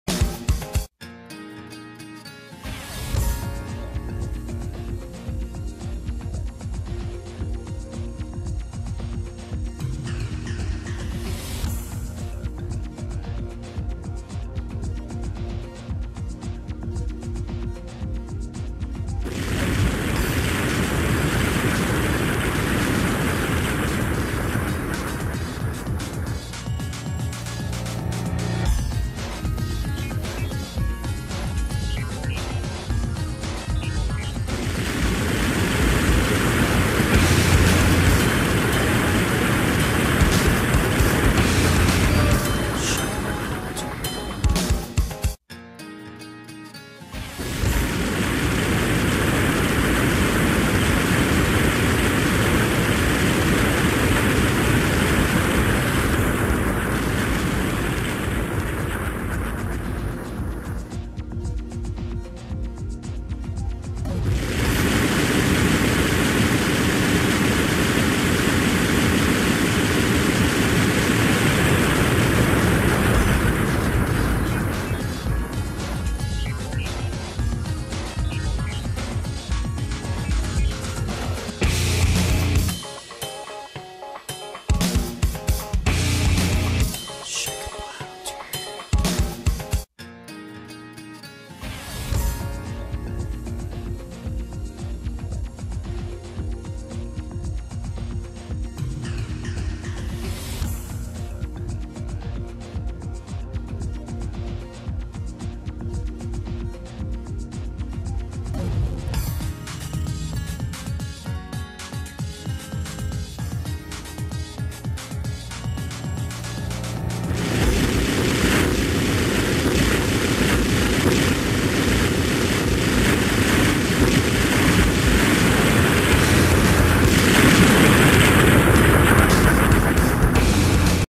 SFx Space Shooter Test 11 Explosions!
I forgot to include explosions in the last video so here they are!